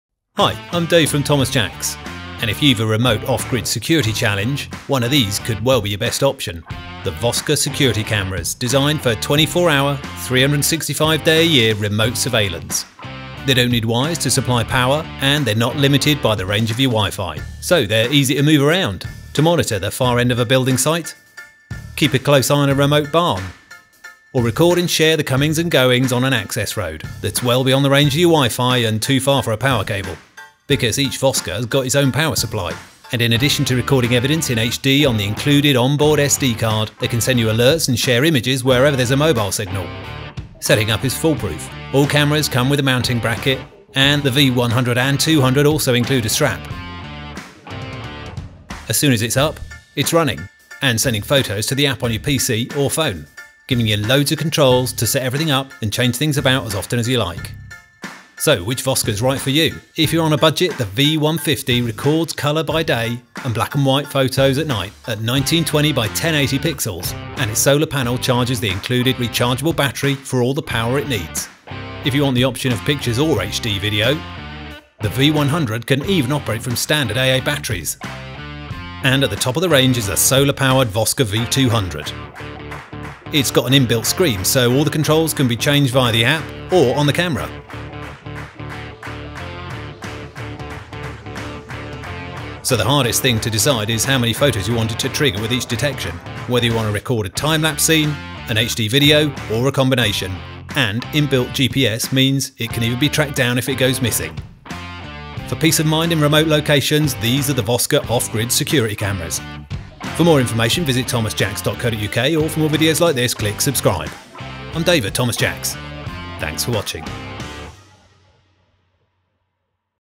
What’s the difference between the Vosker V100, V150 & V200 cellular security cameras?
If your security needs are remote, here’s the Vosker range of security cameras and how you decide whether to choose the V100, V150, or V200.

They all let you monitor security on your phone and they’re not constrained by wires or your wifi network because they’re cellular.

00:00 - Why you'd need a Vosker
00:28 - Setting up a Vosker is easy
00:51 - Introducing the phone app
00:59 - Vosker V150
01:11 - Vosker V100
01:18 - Vosker V200